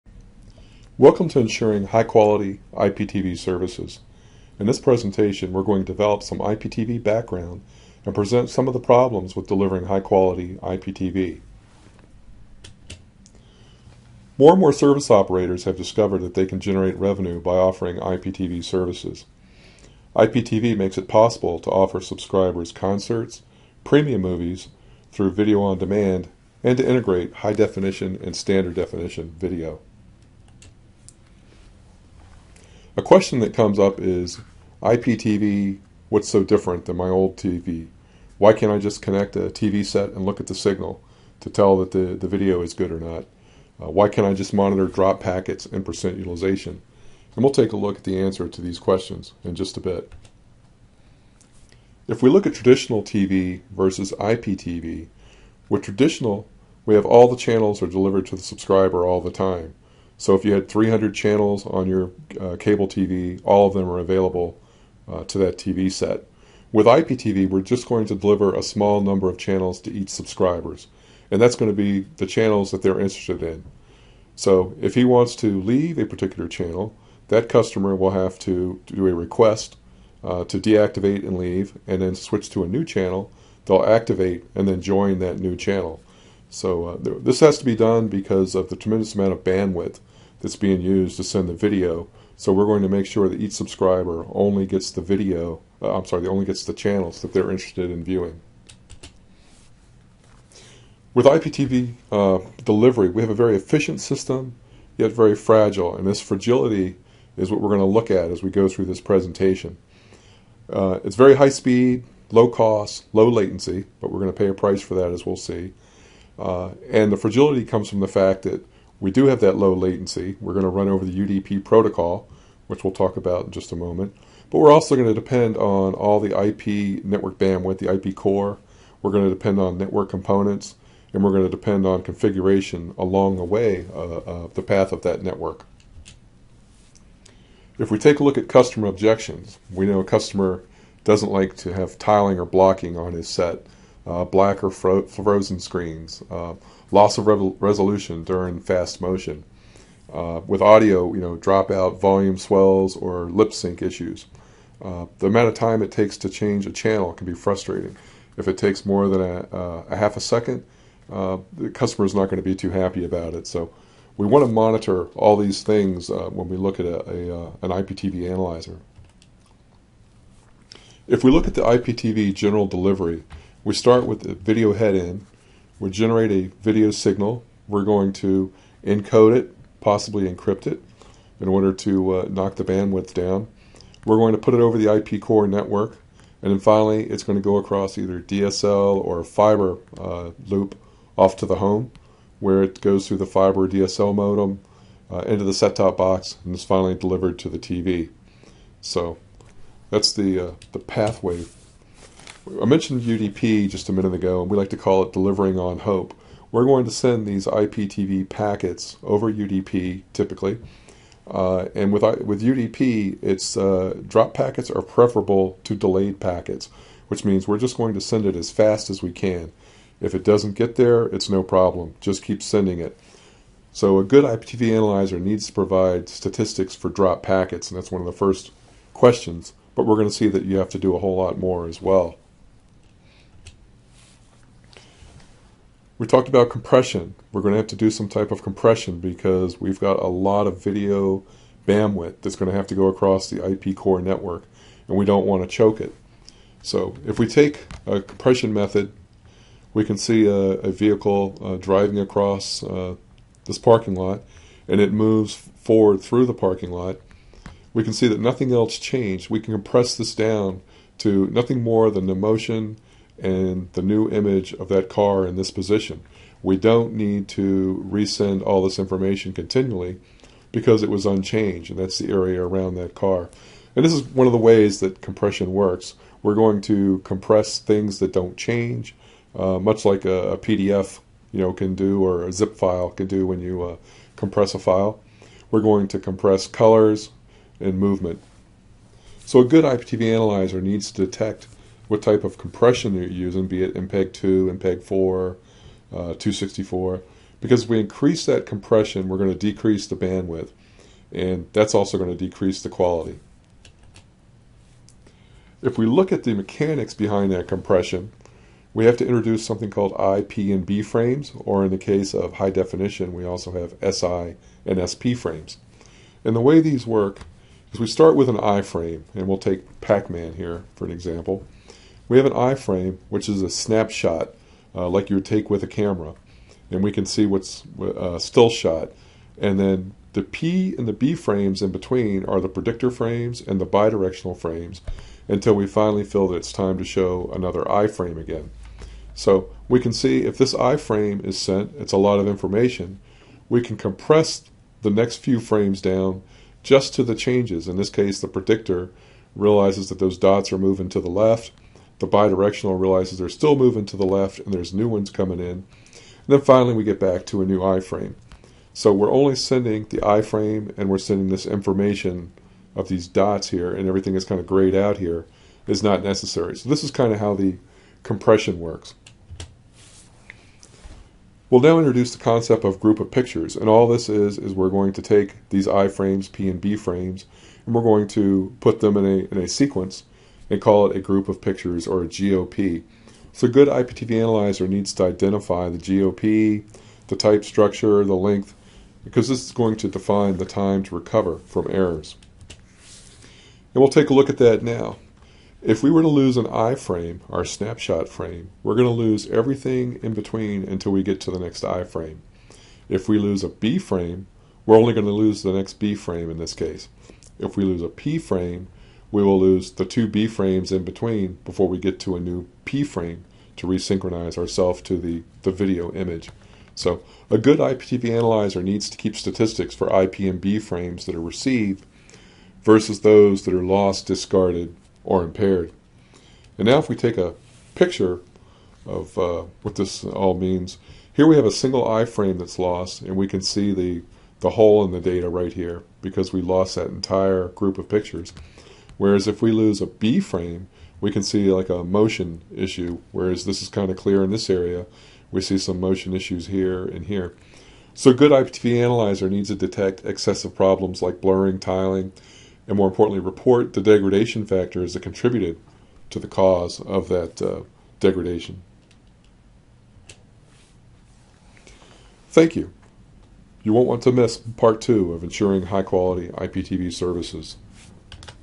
Ensuring High-quality IPTV Services - Part 1
Part 1: Brief tutorial on IPTV and the problems with delivering high quality IPTV